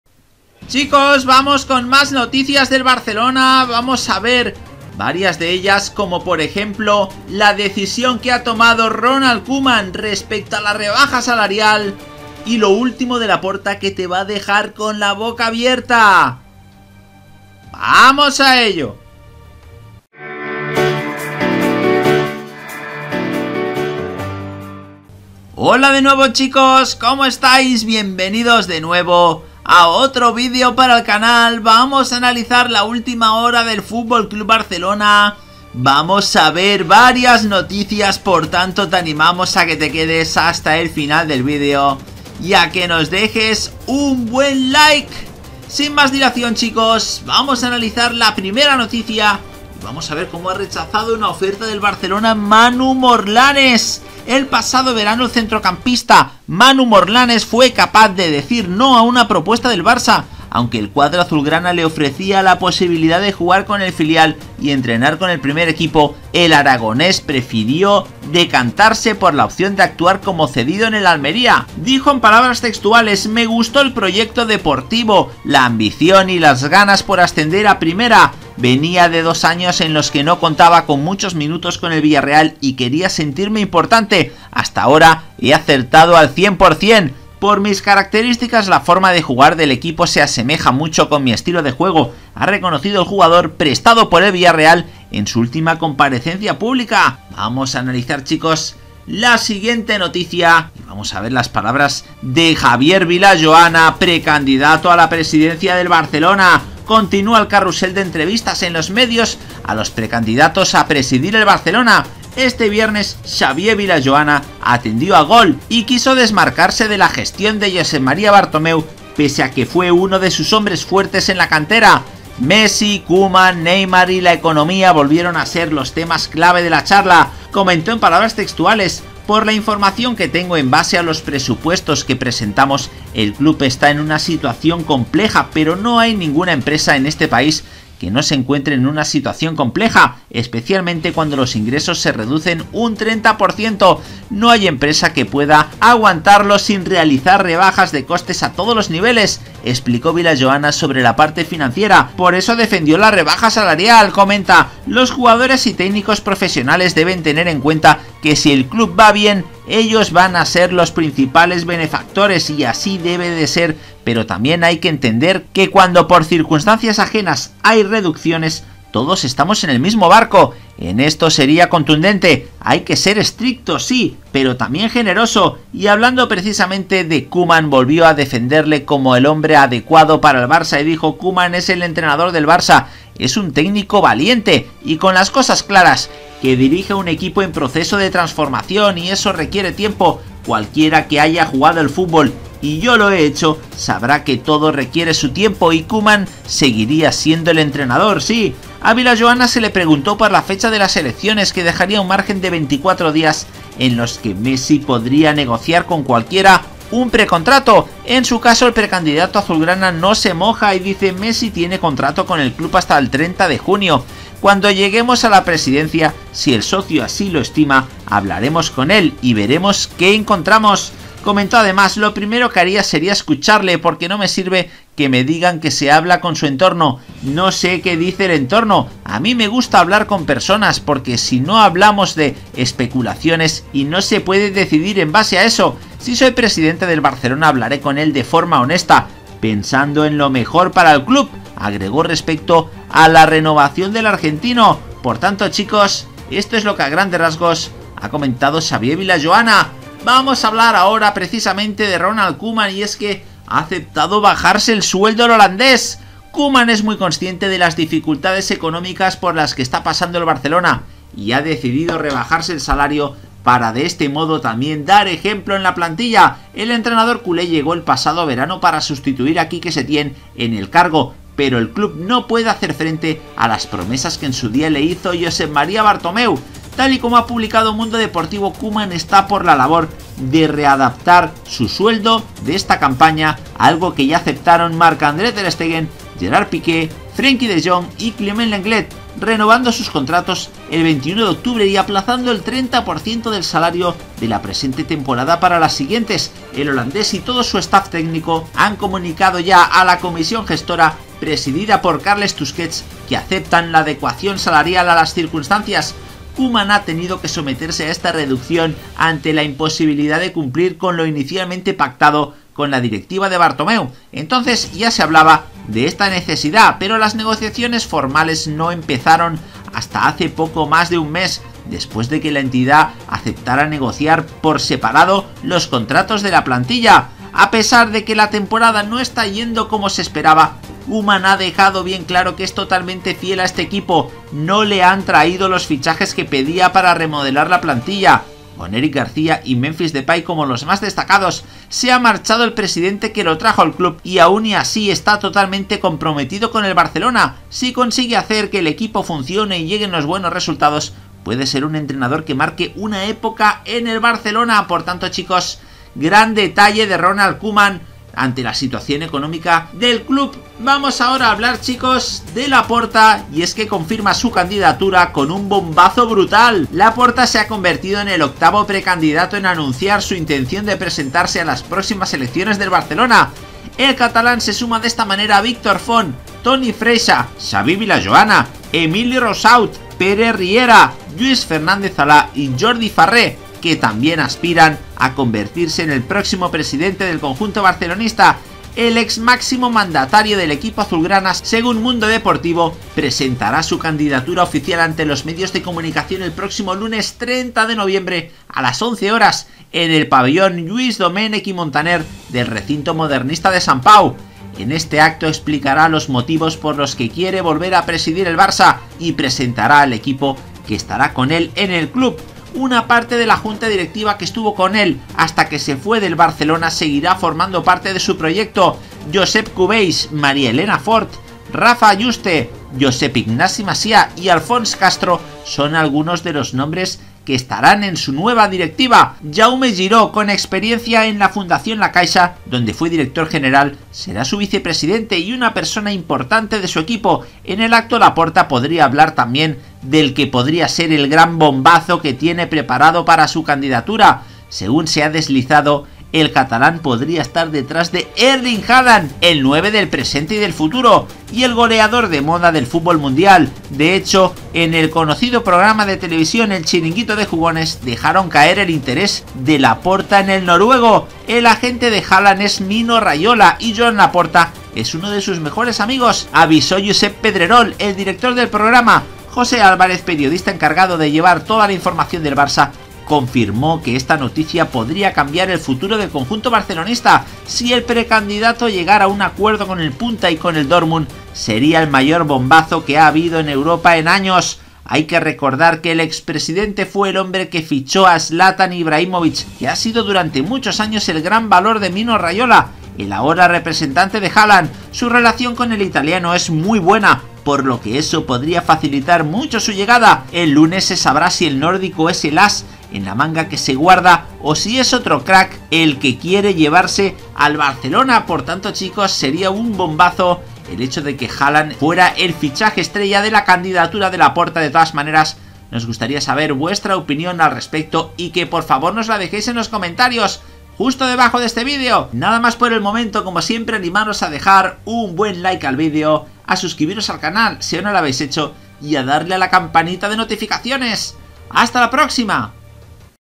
🟥 B0MBAZO: ANUNCIO IMPORTANTE sobre JOAN LAPORTA ⚽KOEMAN sorprendió a todos 🏮 MANU MORLANES 🔴
En este video detallamos varias noticias del FC Barcelona, como el gran bombazo que tiene preparado Joan Laporta de cara a las elecciones del FC Barcelona, la decisión que ha tomado Ronald Koeman y su cuerpo técnico sobre la rebaja salarial, las palabras del precandidato Xavi Vilajoana sobre la masa slarial de la primera plantilla, y lo ultimo sobre el mercado de fichajes en clave Barça.

Fuentes:
besoccer
fcbarcelonanoticias
fichajes net
fichajes com